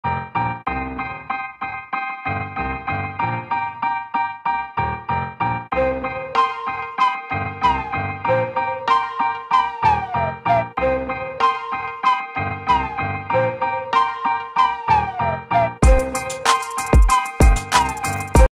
Lego Marvel Iron Spider-Man Brickheadz stop motion build
This stop motion was made with a IPhone and a brick built tripod the animation has 243 frames and was filmed at 12 fps